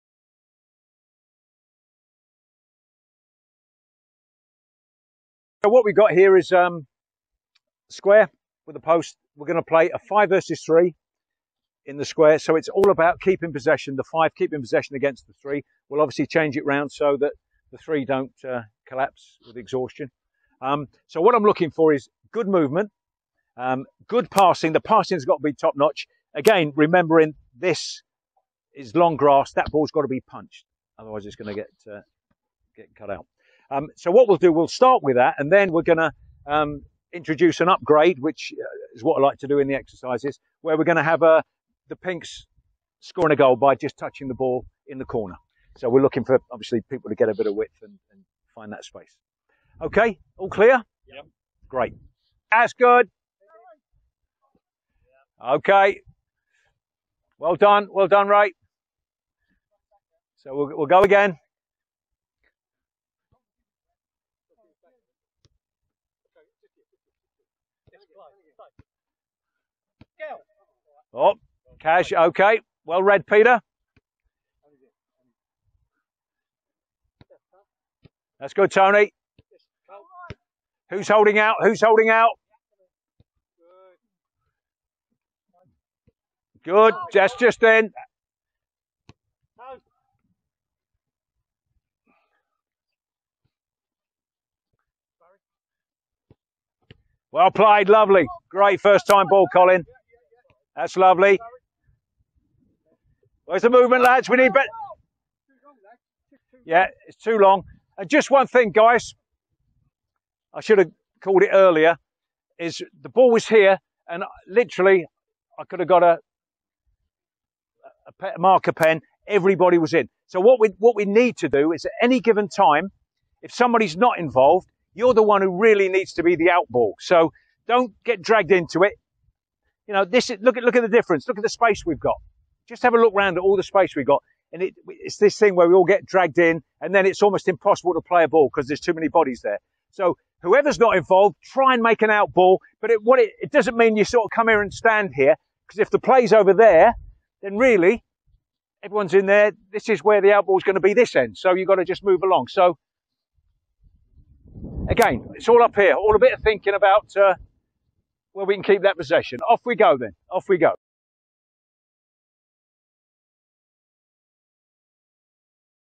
Chelmsford City Walking Football Coaching 3
This video is part of a coaching project being produced at Chelmsford City Walking Football for club members